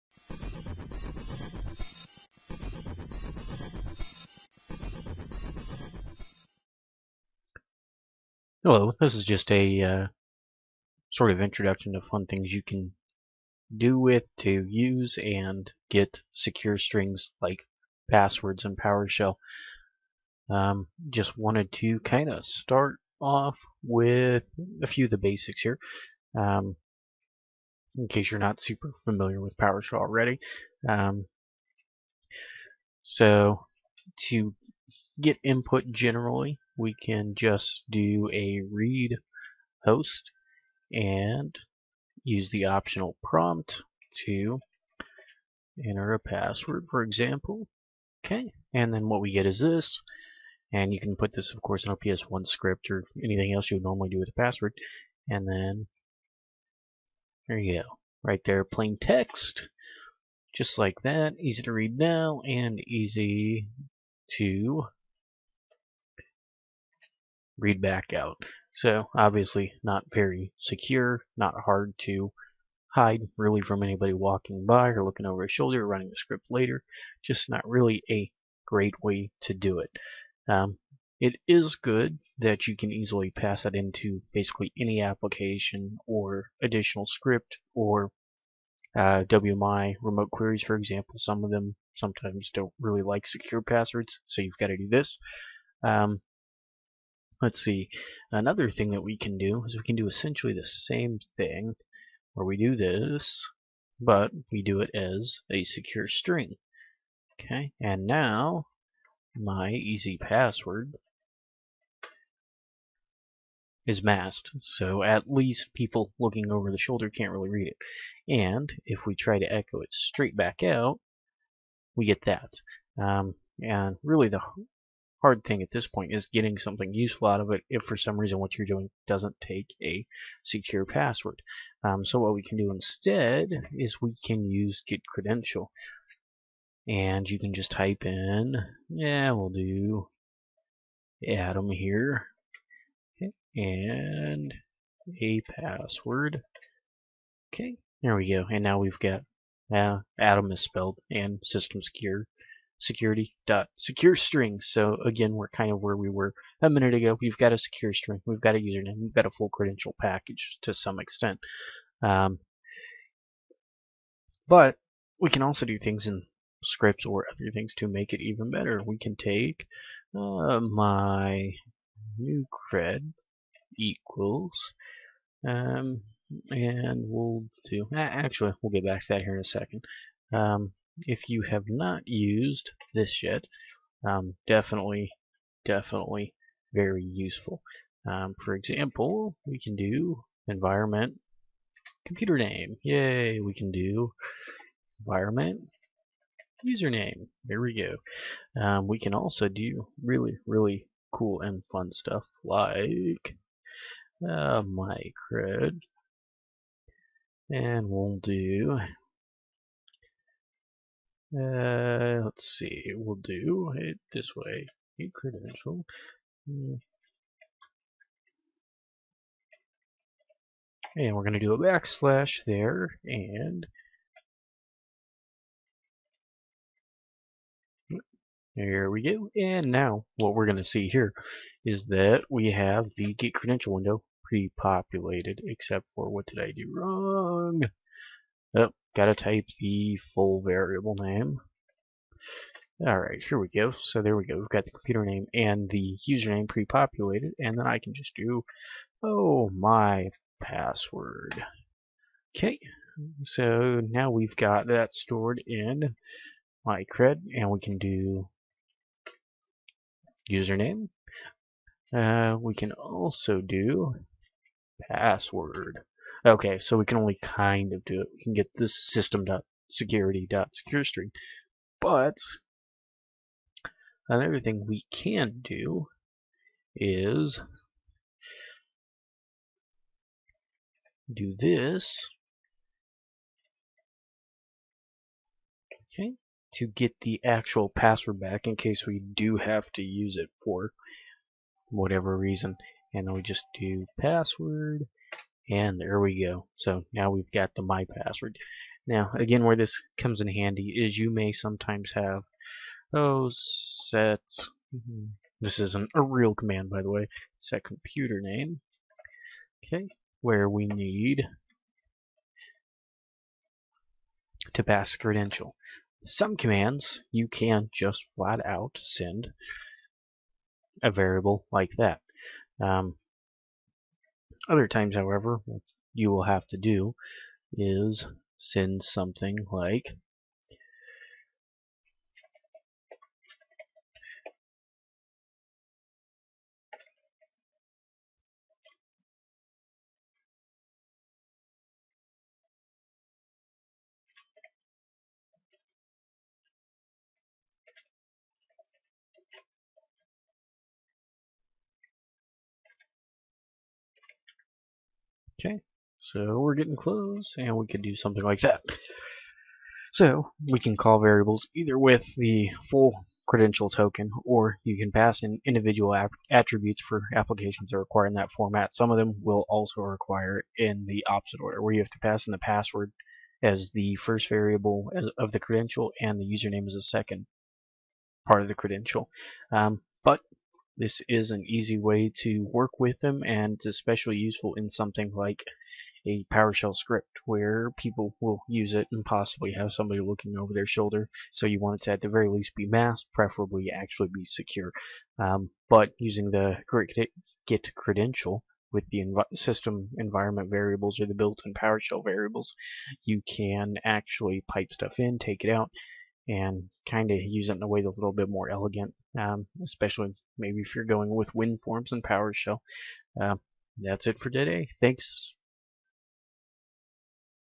How to use Passwords and SecureStrings in PowerShell
I go over using SecureString (System.Security.SecureString) and passwords in PowerShell as well as how to decrypt passwords from Get-Credentials